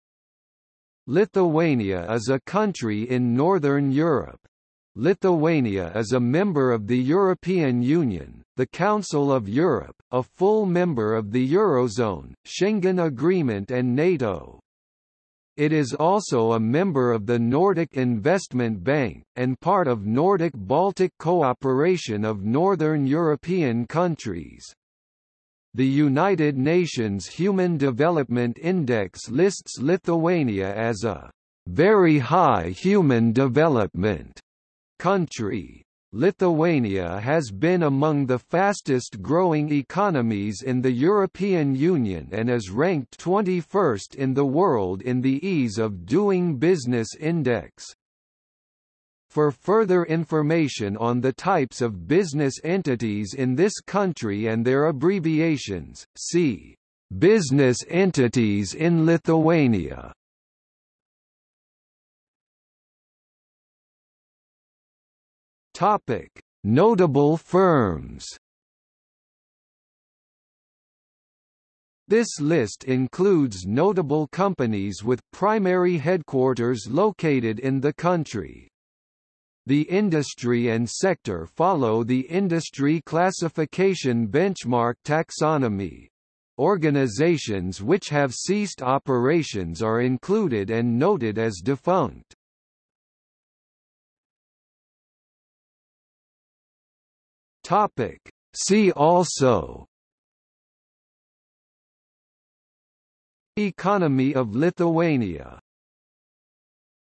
List of companies of Lithuania | Wikipedia audio article
00:01:02 Notable firms
00:01:34 See also

- Socrates

SUMMARY
Lithuania is a country in Northern Europe. Lithuania is a member of the European Union, the Council of Europe, a full member of the Eurozone, Schengen Agreement and NATO. It is also a member of the Nordic Investment Bank, and part of Nordic-Baltic cooperation of Northern European countries. The United Nations Human Development Index lists Lithuania as a "very high human development" country. Lithuania has been among the fastest growing economies in the European Union and is ranked 21st in the world in the Ease of Doing Business Index.
For further information on the types of business entities in this country and their abbreviations, see "Business entities in Lithuania".